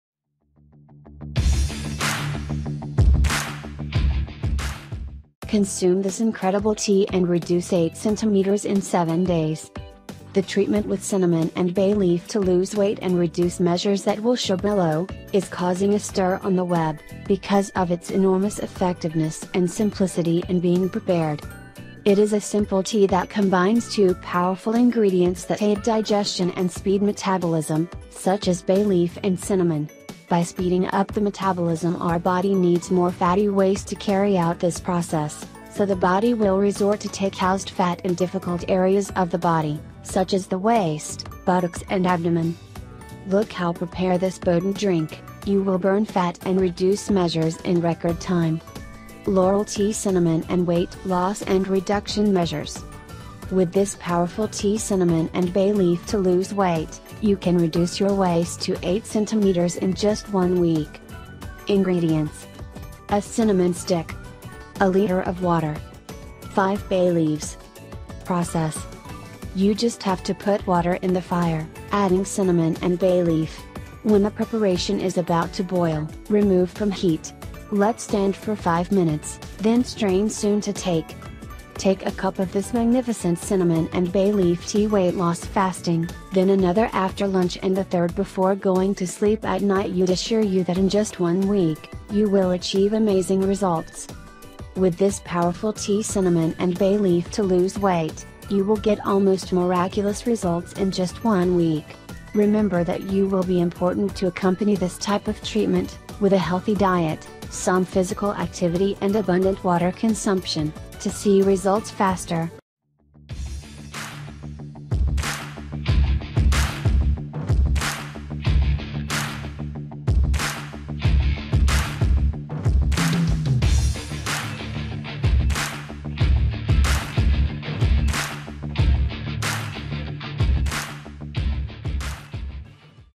Consume This INCREDIBLE TEA and Reduce 8 cm in 7 DAYS How To Reduce Waist Fat
i explained in this video
The remedy with cinnamon and bay leaf to shed pounds and reduce measures in order to show underneath, is inflicting a stir at the net, due to its widespread effectiveness and simplicity in being prepared.